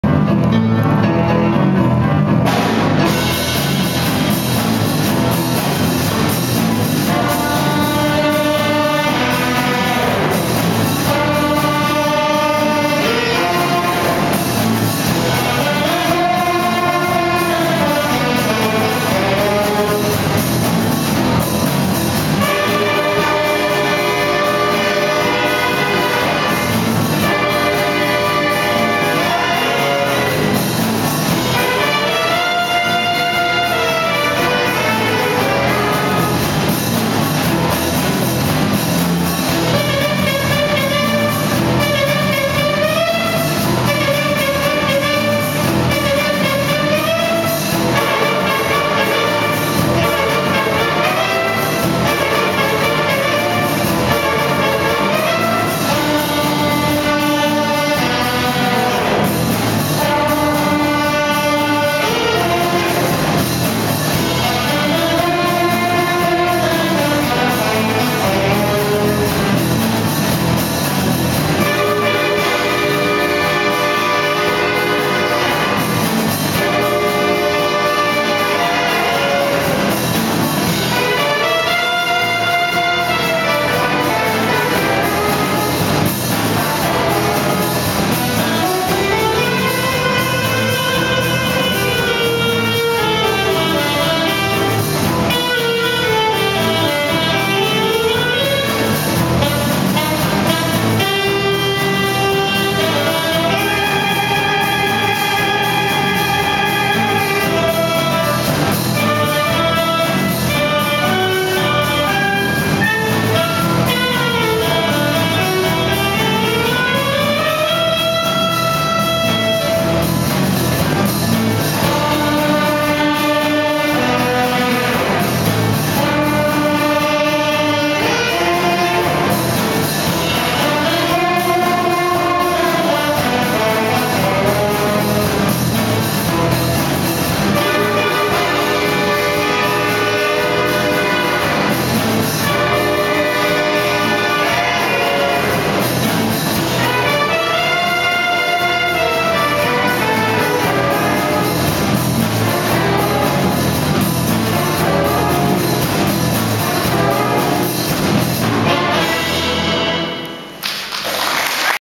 Симфо - джаз оркестр студентів МДПУ ім. Б. Хмельницького
Всеукраїнська студентська олімпіада зі спецальності "Музичне мистецтво", Мелітопольський ДПУ ім. Б. Хмельницького, Мелітополь, 25-27 квітня 2016 р.